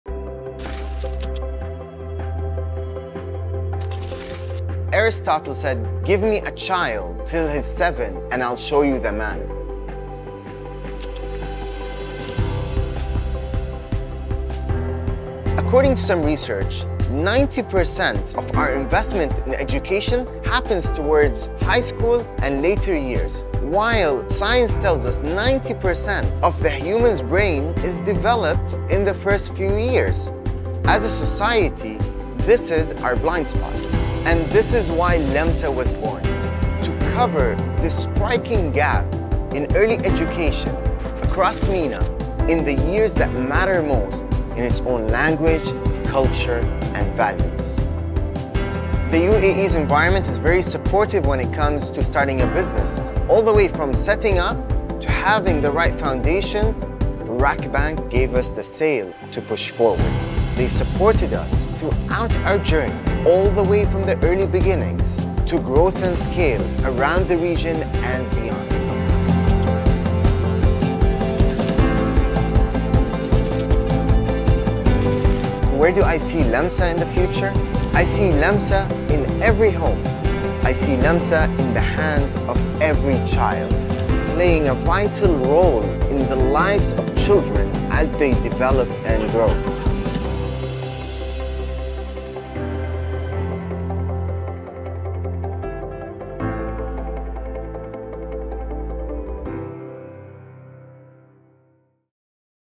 Lamsa's Sail To Success by RAKBANK Business
From being recognised as one of 'The Most Promising UAE Startups' to expanding across KSA, Egypt & North Africa, Lamsa has truly grown to become the top educational app for kids in the region. Here's the inspiring story of Lamsa's SailToSuccess shared by its founder and our business banking customer, Badr.

Want to sail your startup to success? Know more